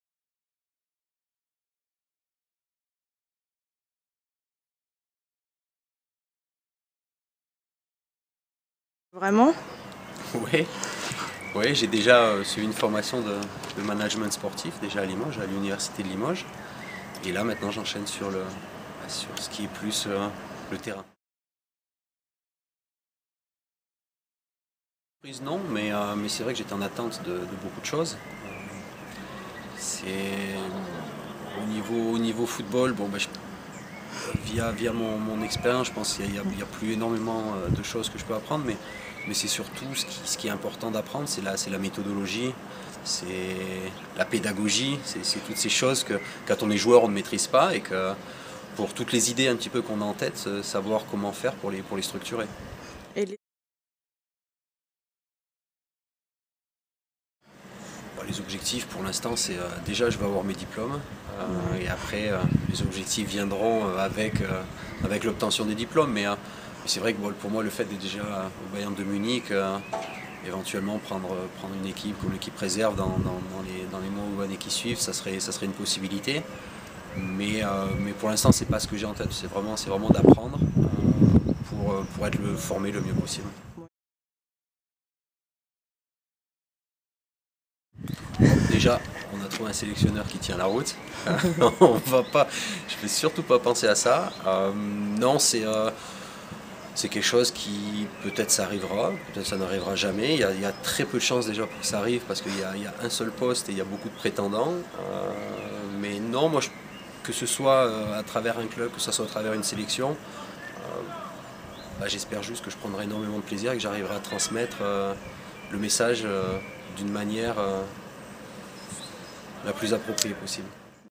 Interview de Willy Sagnol
Willy Sagnol passe ses diplômes d'entraîneur à l'Institut du Football Régional de Châteauroux.